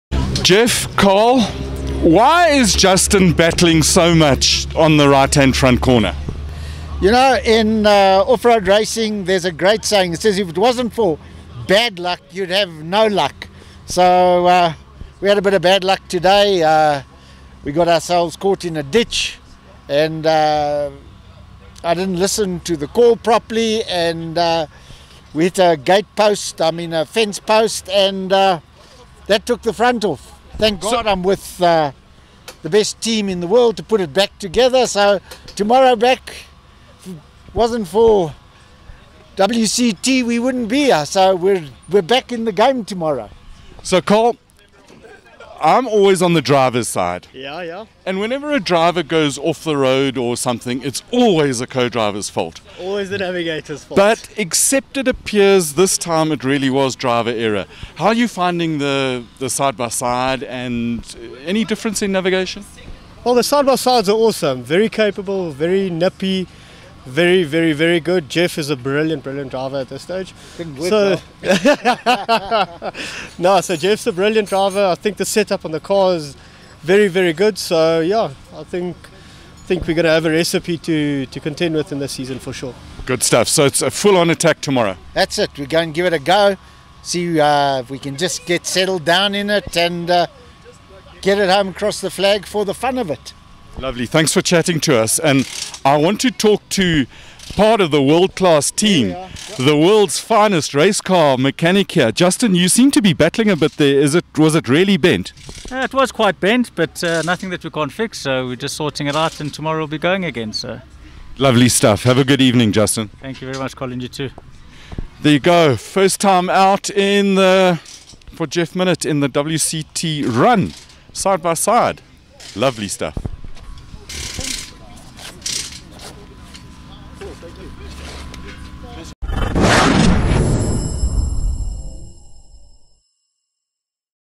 Geoff Minnitt and Carl Swanepoel in the G07 SxS on the Mpumalanga 400
Geoff Minnitt and Carl Swanepoel had a troubled Mpumalanga 400 in Dullstroom